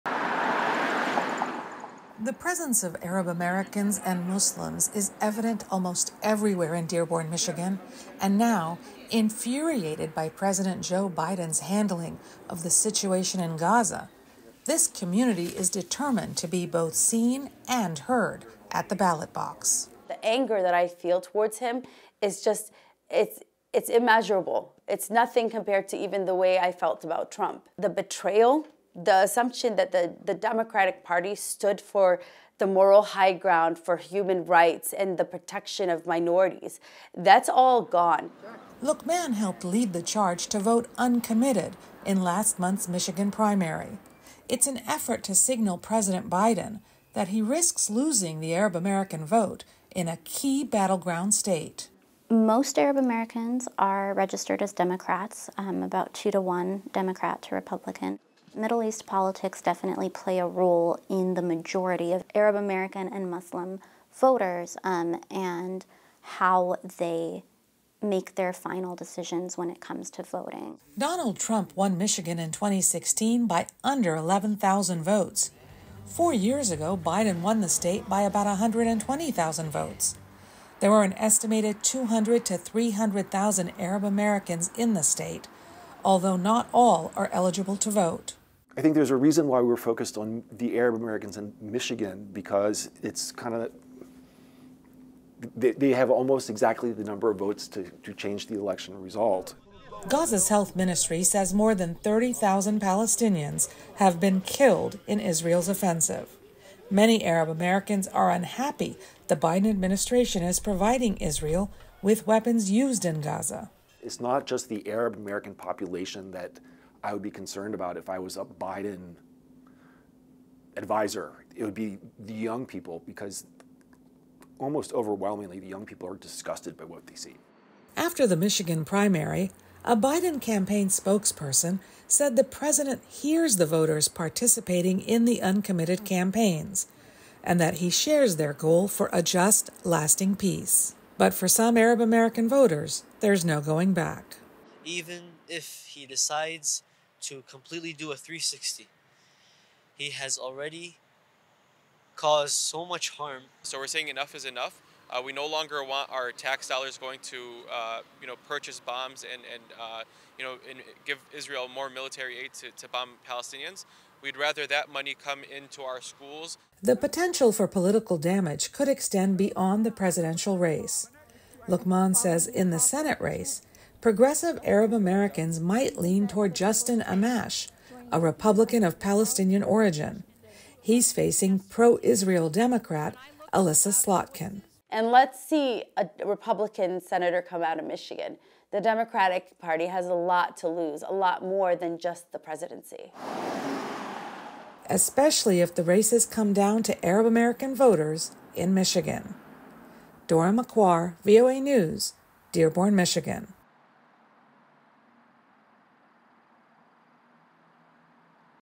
Will Angry Arab American Voters Swing Michigan Away from Biden?
Michigan has the largest Arab American community in the country and they’re furious with the president’s handling of the Israel-Hamas conflict. VOA’s Dora Mekouar reports from Michigan. Camera, video edit: Adam Greenbaum